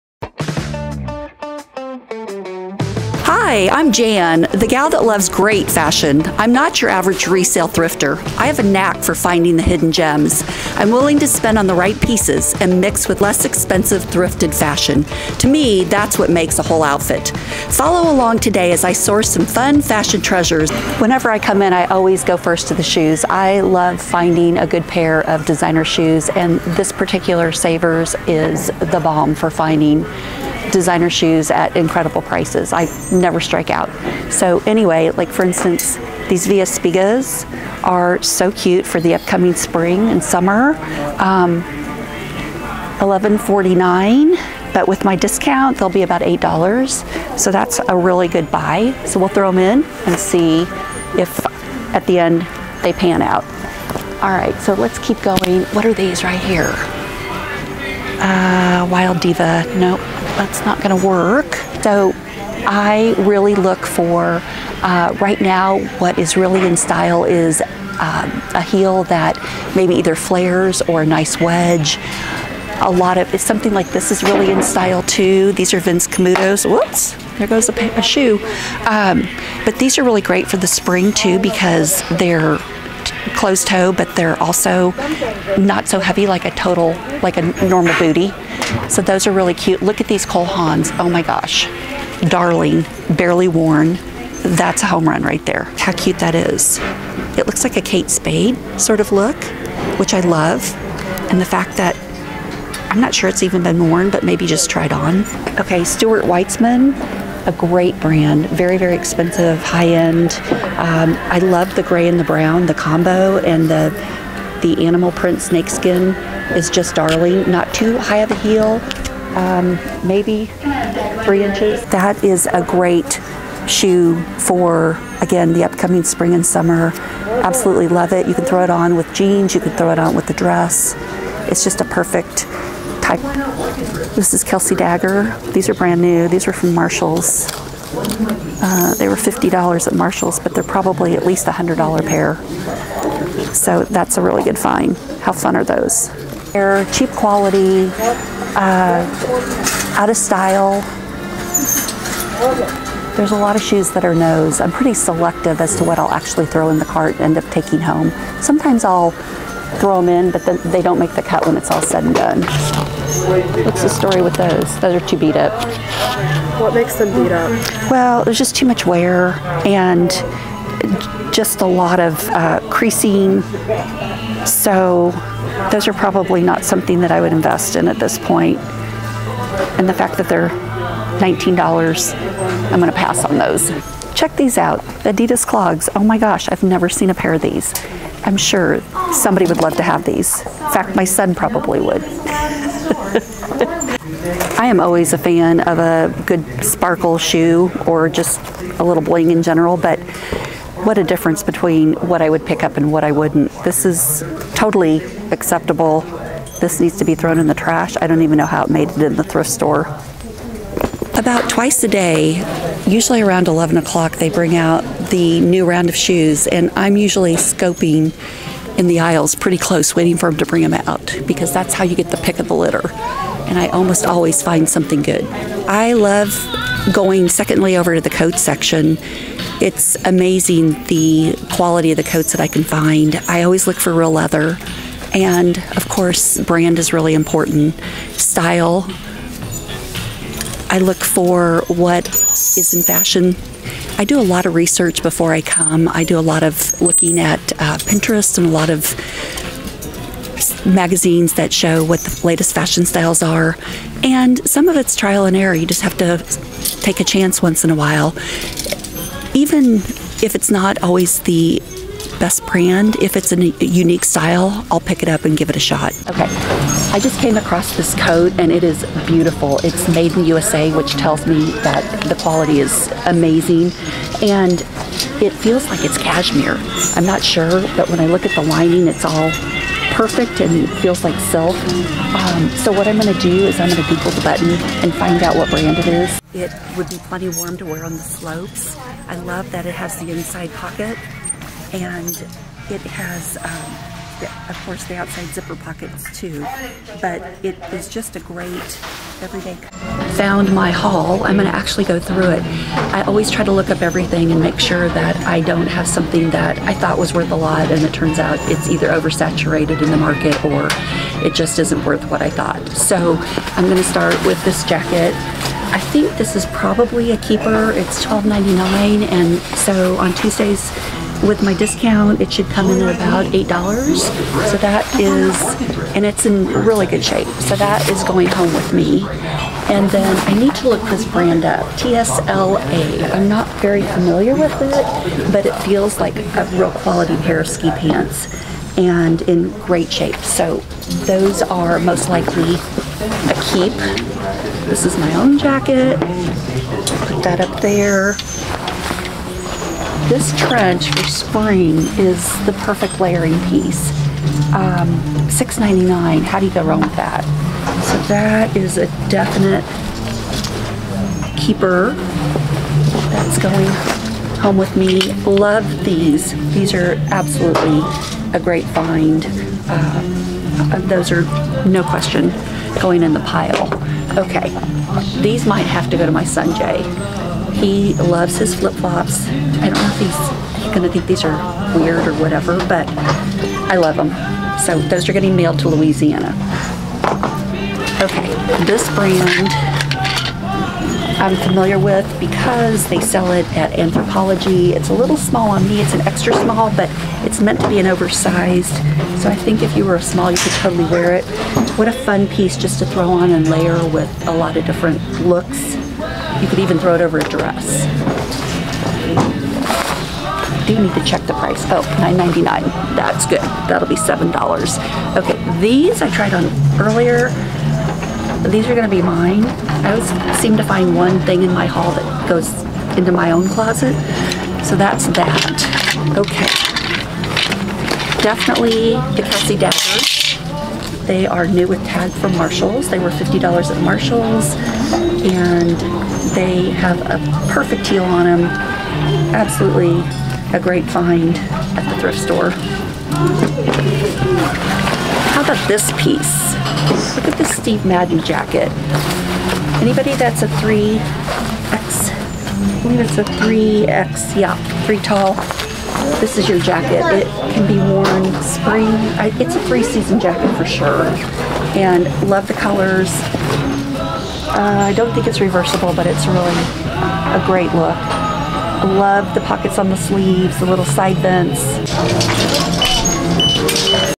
Episode 1: Thrifting 101
Hi!
My name is Jan Long and I am a Midwest thrifter! I love finding deals and now it is time I show you how I work my magic. Follow along on this journey!